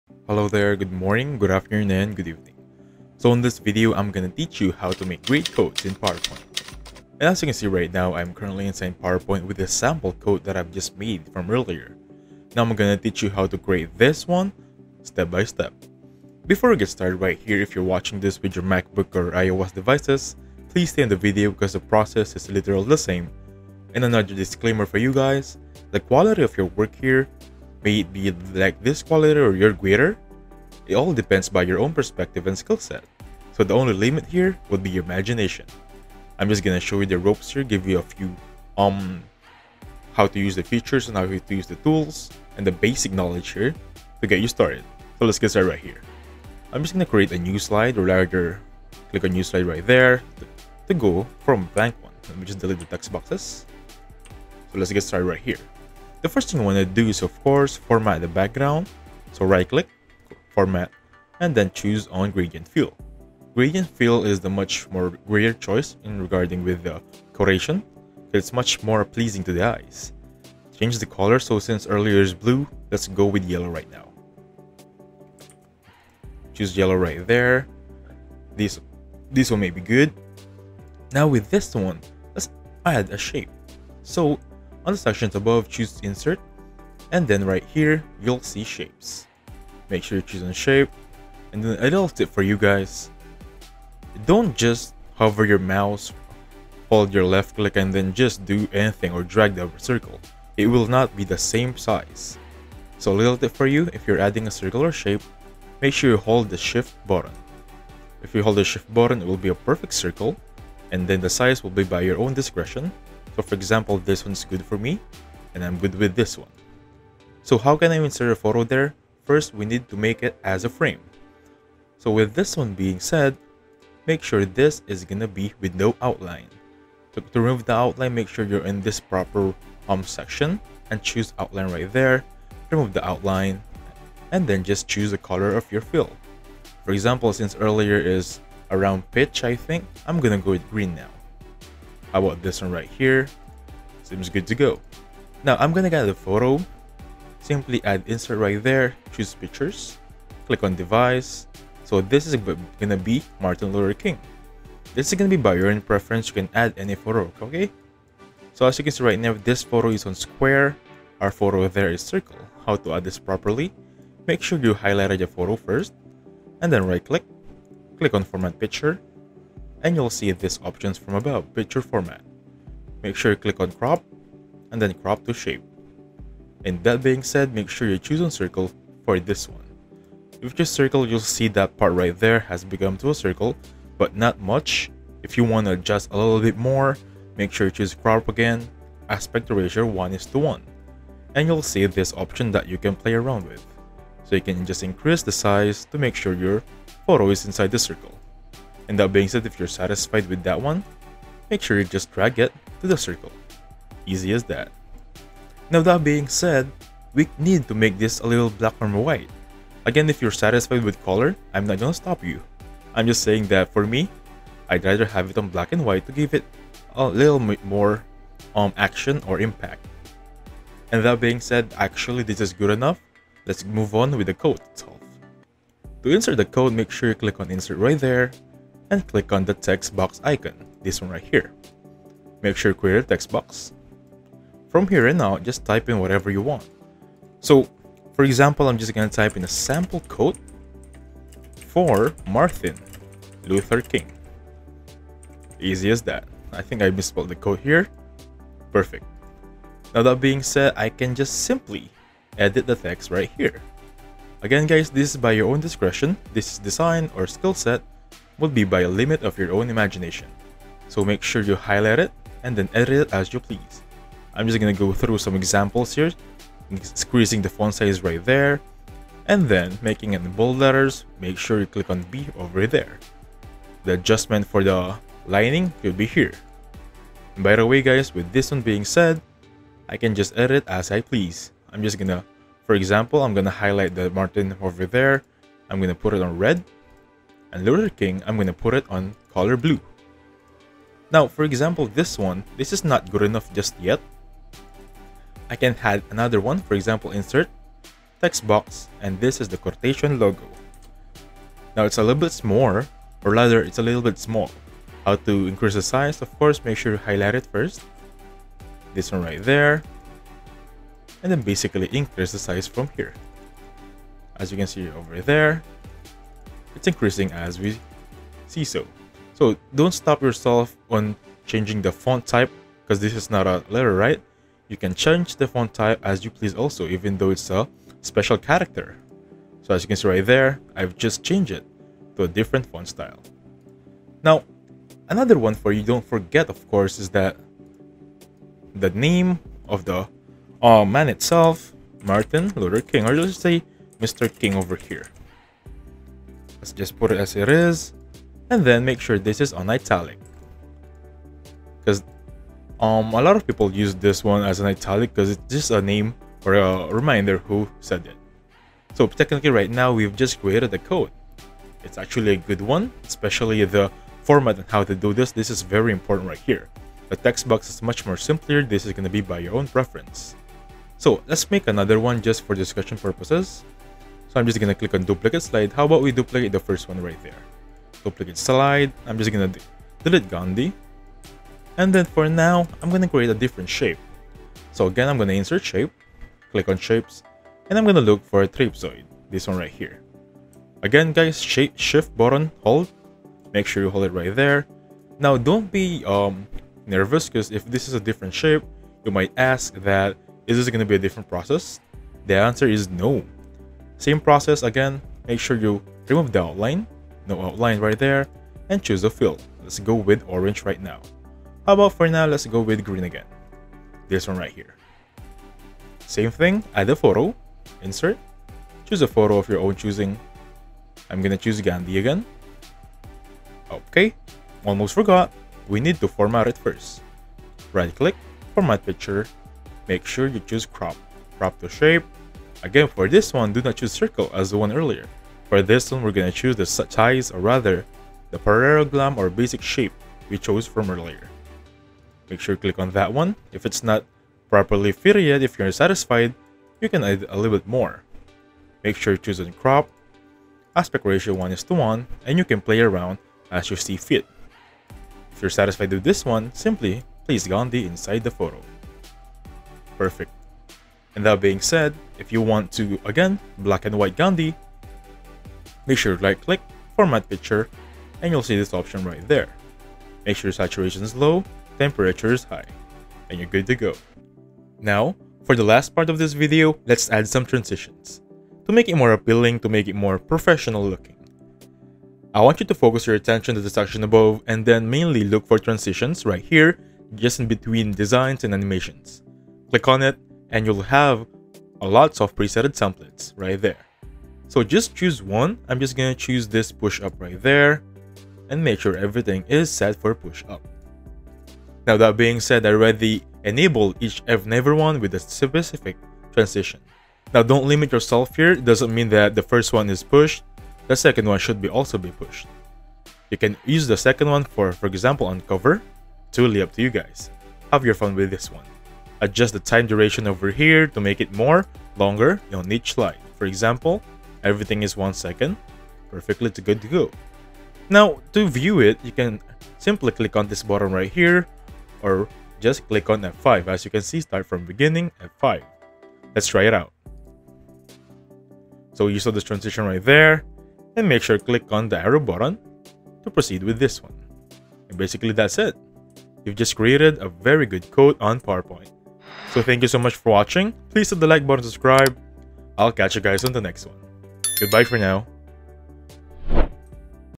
How To Make Great Quotes Slides In PowerPoint
Elevate your PowerPoint presentations with our tutorial on "How To Make Great Quotes In PowerPoint." In this comprehensive guide, we delve into the art of creating impactful and visually appealing quotes using Microsoft PowerPoint. Whether you're a seasoned presenter or a beginner, this tutorial provides valuable tips and tricks for enhancing your slides with motivational and inspiring quotes.

Unleash your creativity as we explore PowerPoint design techniques, animation, and tips for making your quotes stand out. From quote animation in PowerPoint to crafting a visually appealing presentation, this tutorial covers it all.

Empower your presentations with the knowledge of making great quotes in PowerPoint. Whether you're working on a business presentation or a personal project, this tutorial will guide you in creating impactful slides.